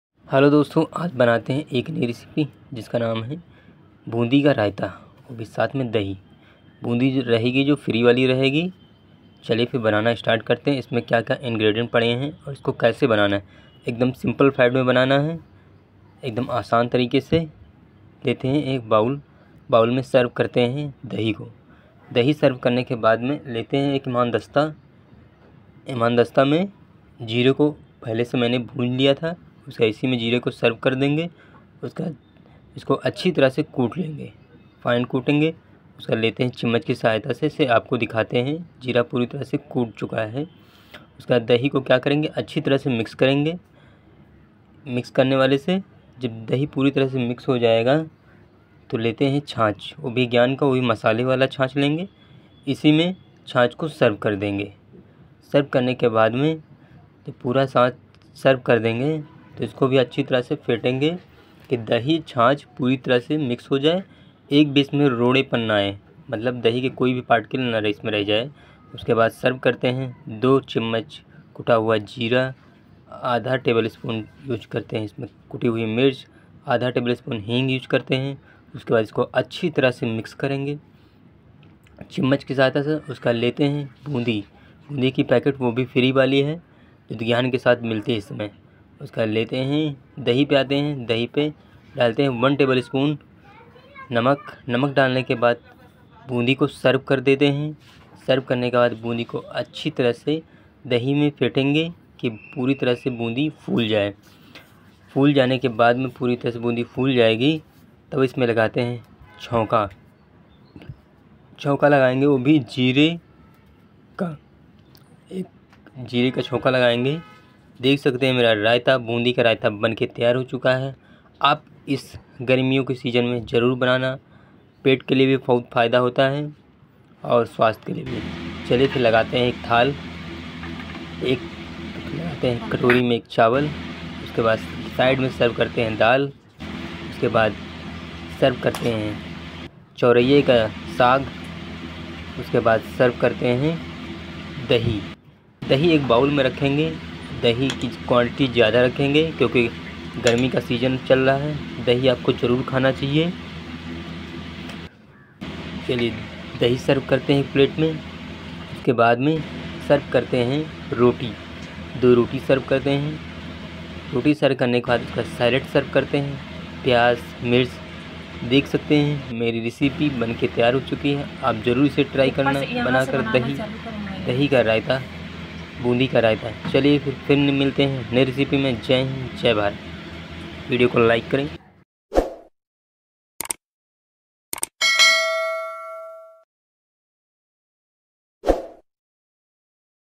बूंदी का रायता ऐसे बनायेंगें तो रोज-रोज बनाकर खायेंगें | Make Tasty Boondi Raita WithSpecial Masala
बूंदी का रायता ऐसे बनायेंगें तो रोज-रोज बनाकर खायेंगें | Make Tasty Boondi Raita With Special Masala